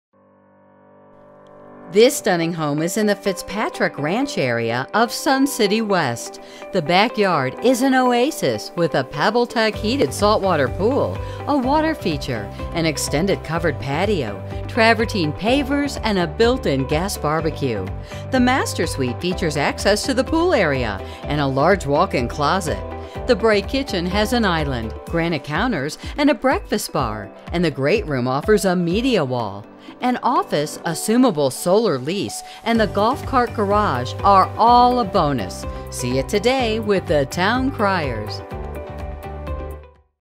14725 W Black Gold Court Sun City West, AZ | ColdwellBankerHomes.com
14725 W Black Gold Court, Sun City West, AZ

Come see this absolutely stunning home in the Fitzpatrick Ranch area of Sun City West, Arizona. The private backyard is an entertainment oasis with a Pebble Tech heated saltwater pool, water feature, extended covered patio, travertine pavers and built-in gas BBQ. If you like to entertain, in the Sun